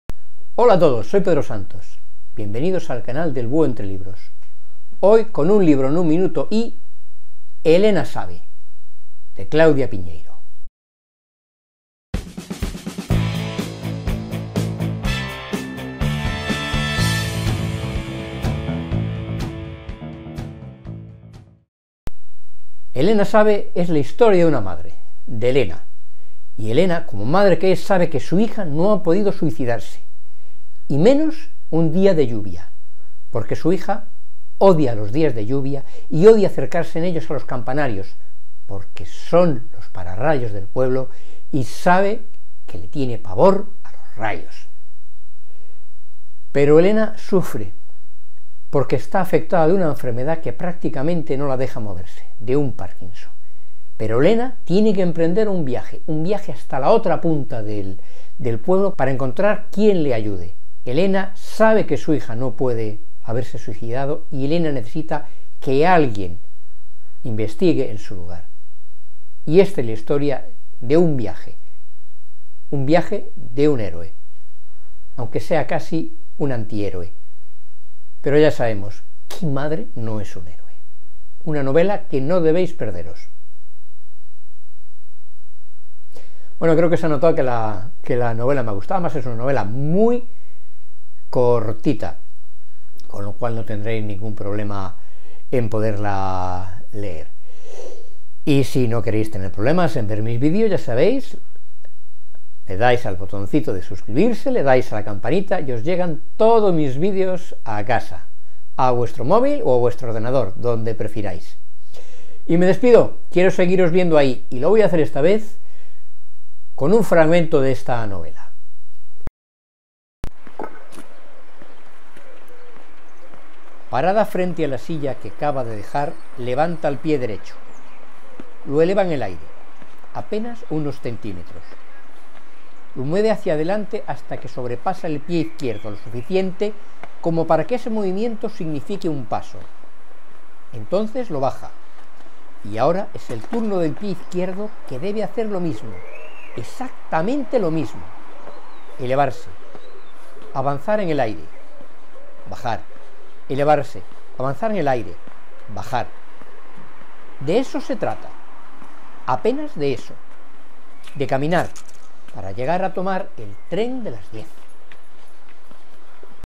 Un libro en un minuto: ELENA SABE (Claudia Piñeiro)
Breve vídeo reseña de "Elena sabe", breve novela de Claudia Piñeiro. La historia de una madre muy afectada de Parkinson en busca de la verdad sobre su hija, presuntamente suicidada en el campanario de la iglesia.